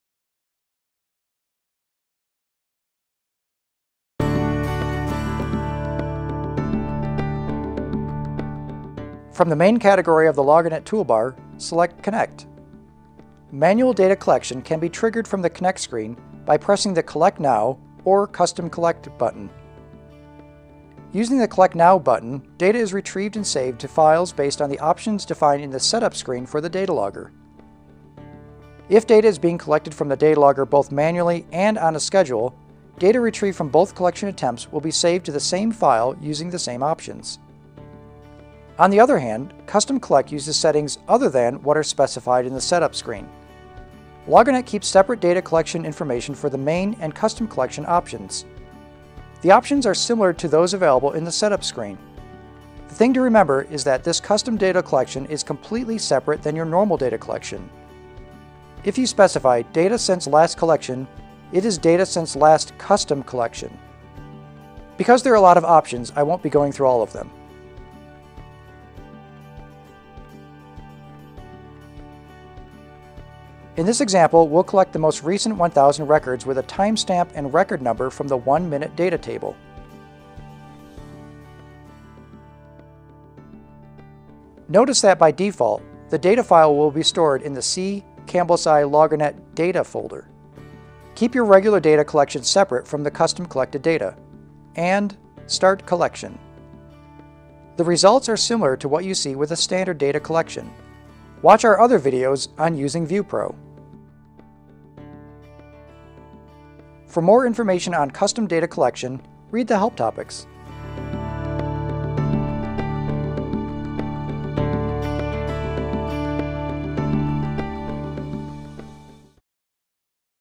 Connect | Custom Collect
LoggerNet’s Custom Collection window lets you set specific options for a manual data collection attempt.